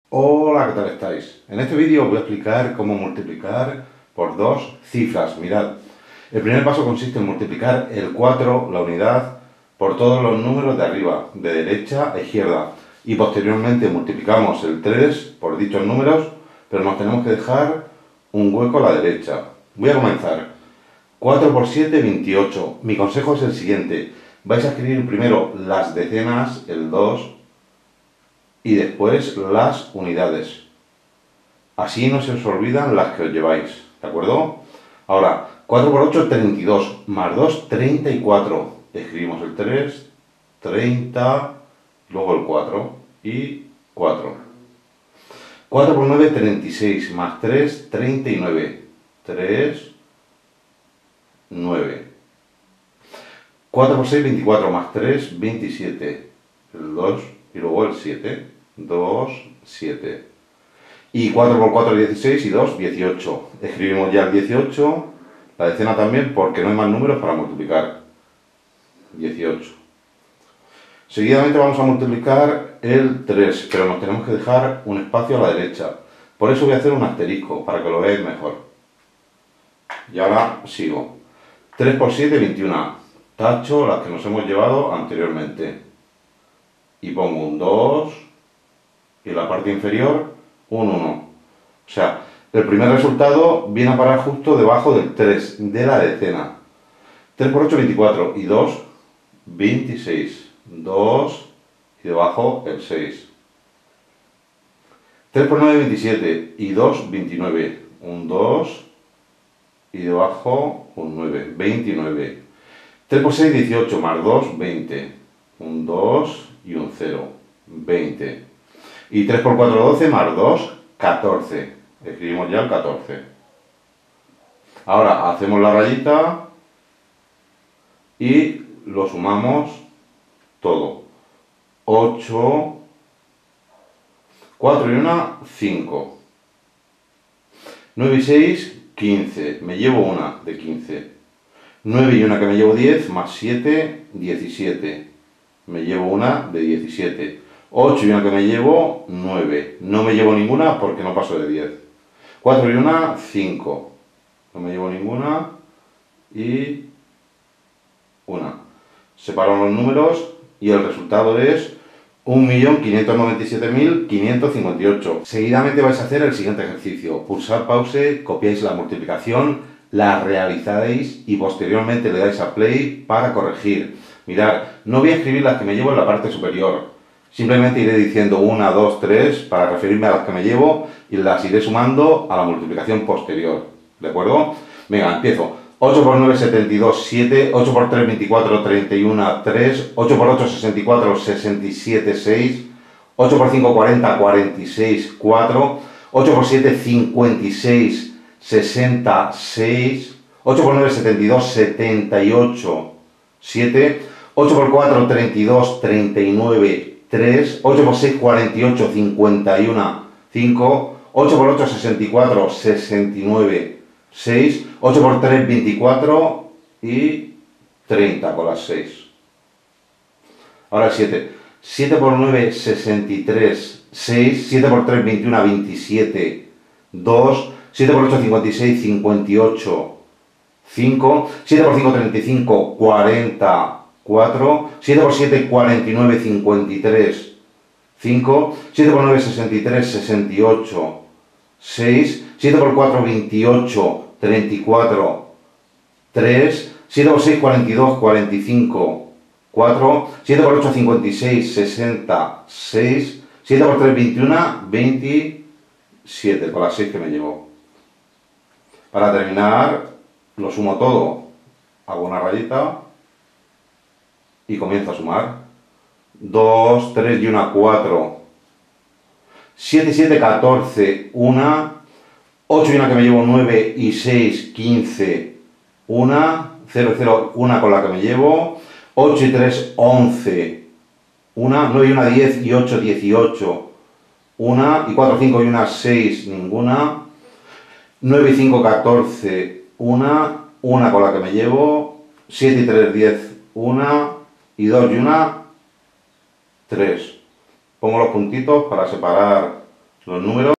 Multiplicación por dos cifras
Aprender a multiplicar por dos cifras o dígitos.